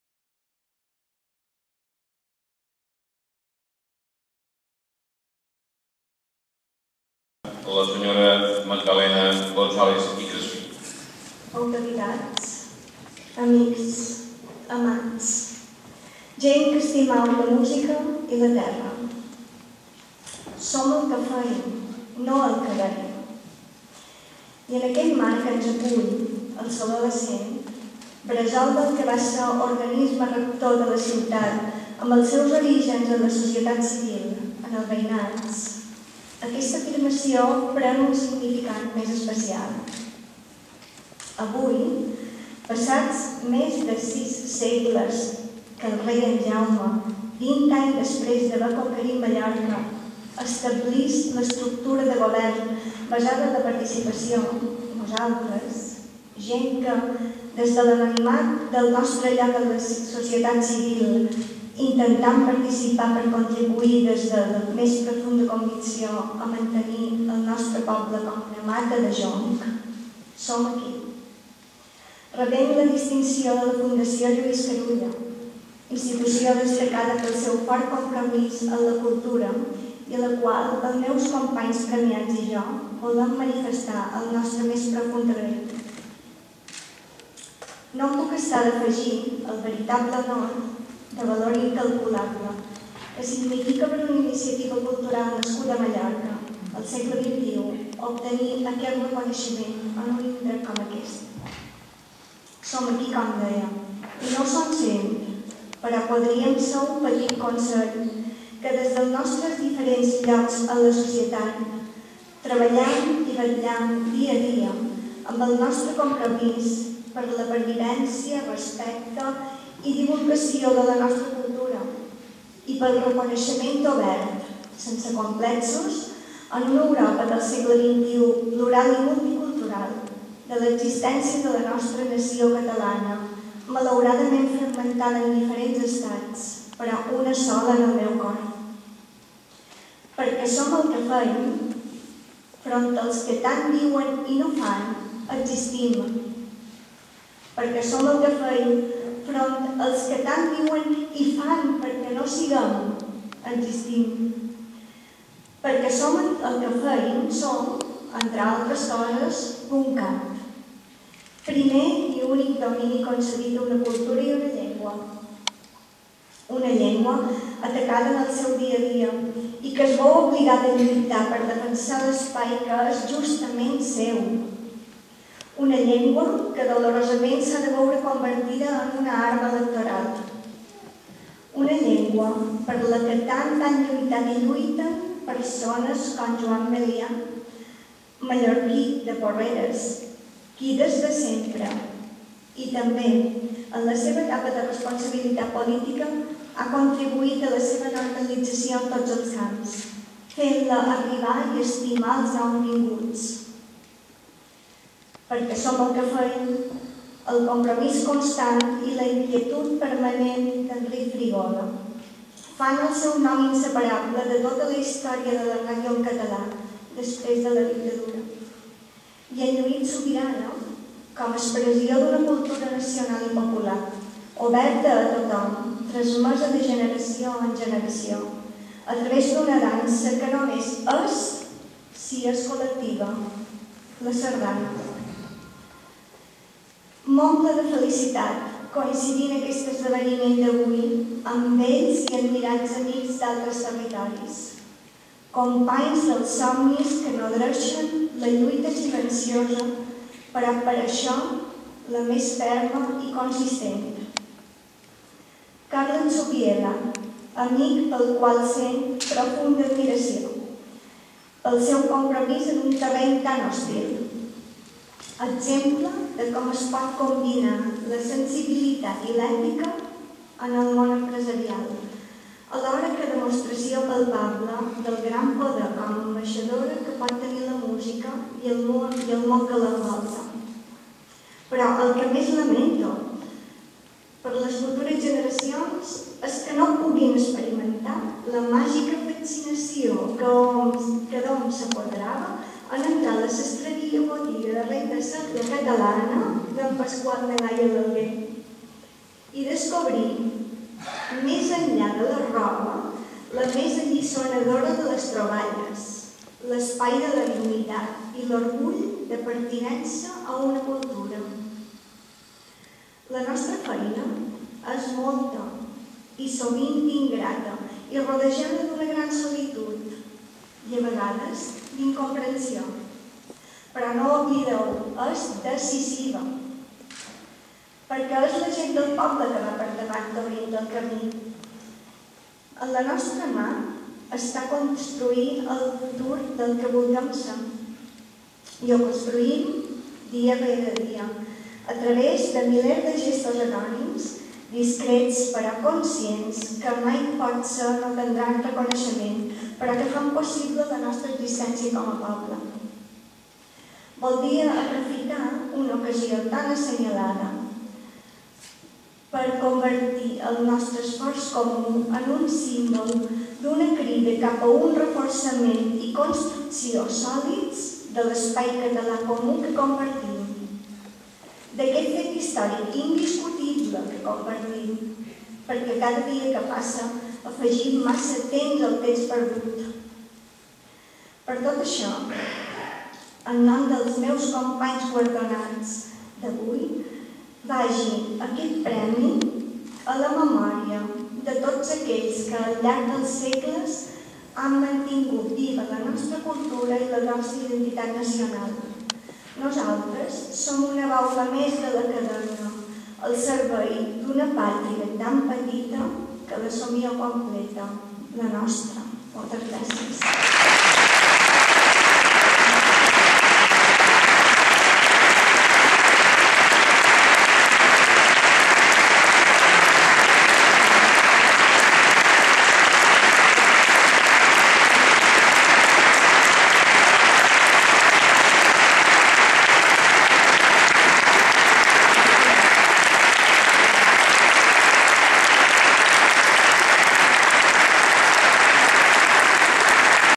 Magda González, Premi d'Actuació Cívica 2010 (a)
Imatges i sons enregistrats durant el discurs que Magda González Crespí pronuncià al Saló de Cent de l'Ajuntament de Barcelona, durant el lliurament dels XXVIII Premis d'Actuació Cívica que la Fundació Lluís Carulla li atorgà, a ella i a cinc persones més que "actuen al servei de la identitat nacional catalana", el 18 de novembre de 2010